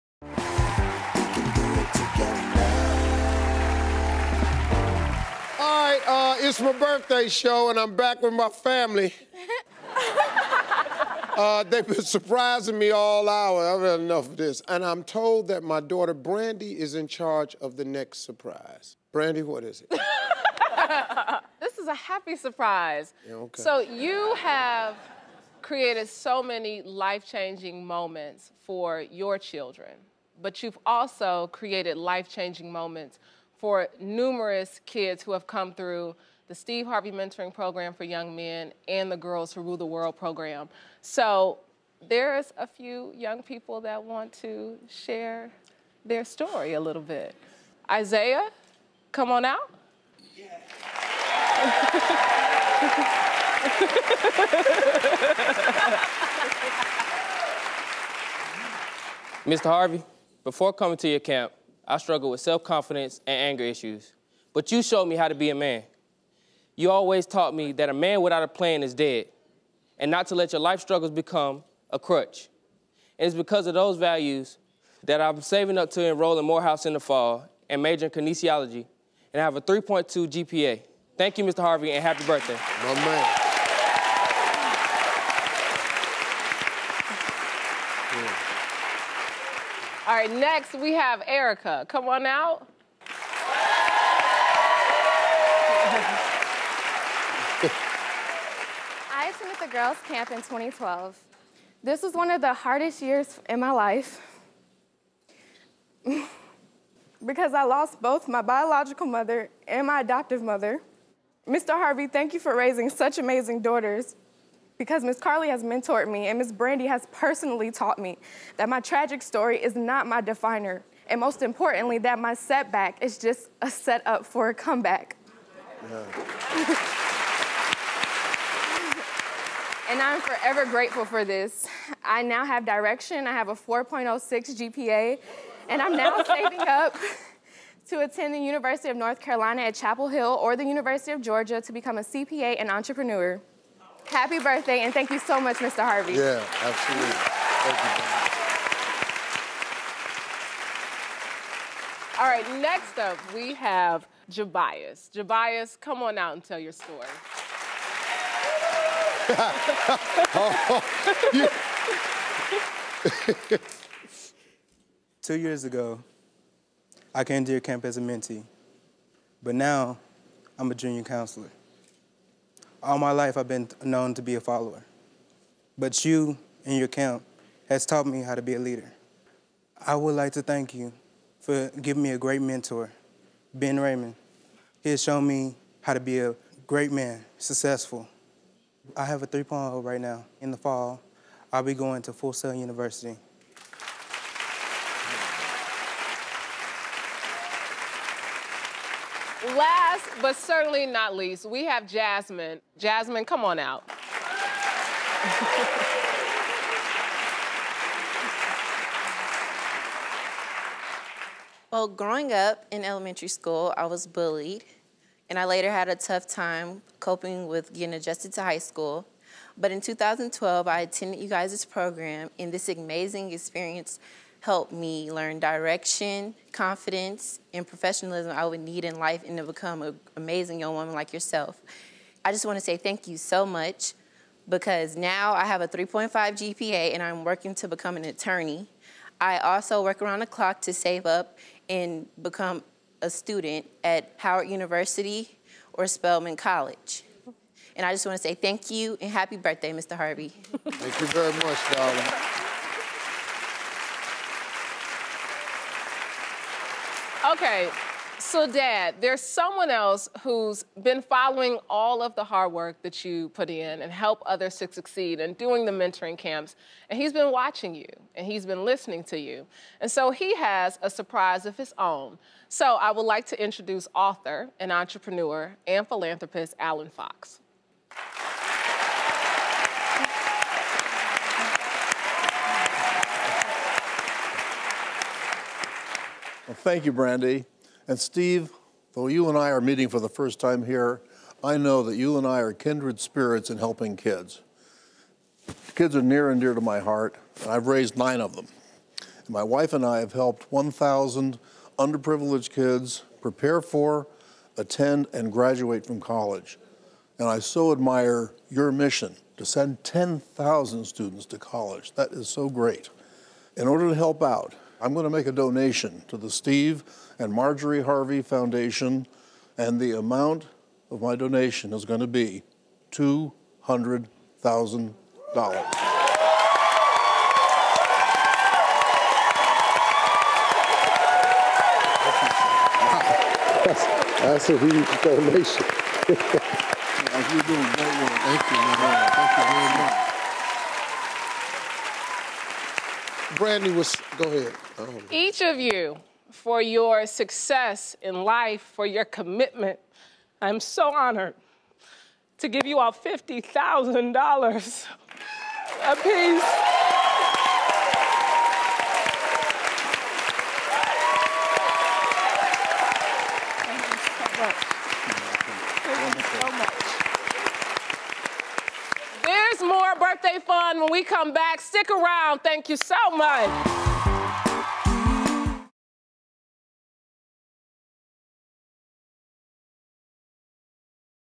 A Surprise Steve Harvey Never Saw Coming
Four children who have received help from the Steve and Marjorie Harvey Foundation surprised Steve to say thank you in person. Philanthropist, businessman and NY Times bestselling author Alan Fox surprised Steve and Marjorie with a huge donation for their foundation.